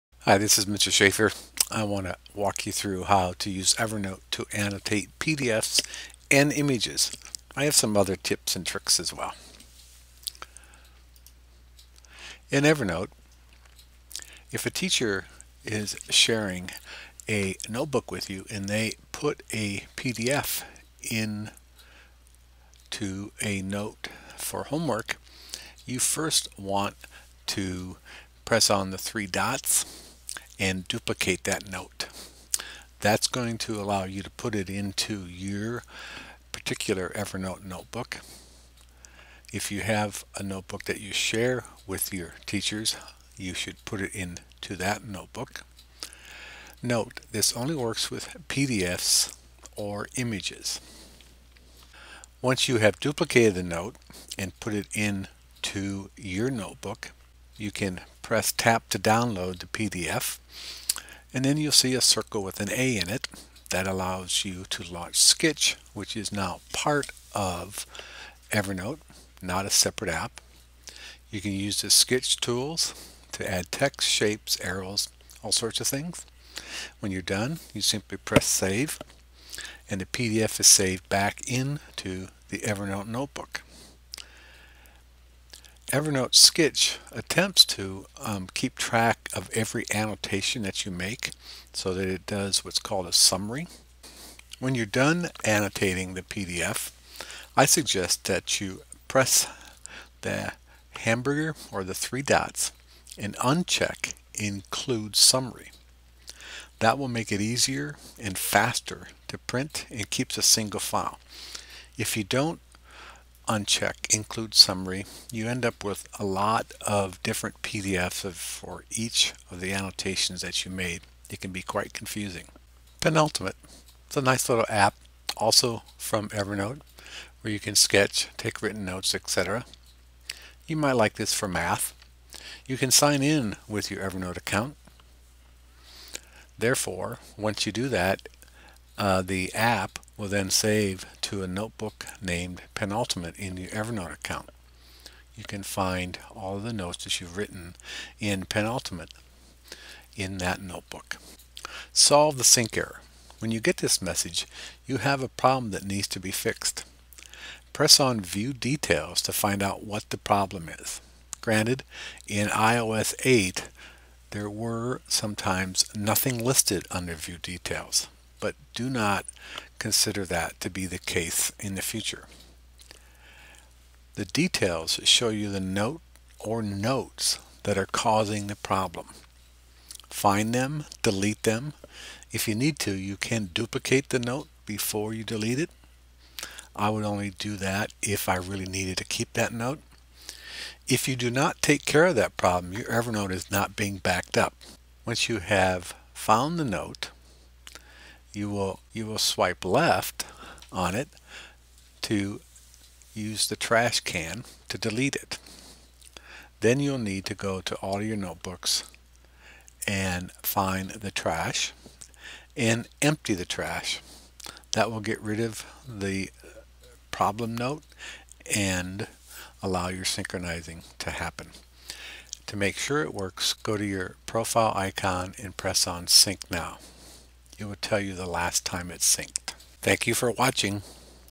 Annotate PDFs or Images and Other Evernote Tips 2017
Learn how to annotate PDFs or Images along with how to solve syncing errors in Evernote.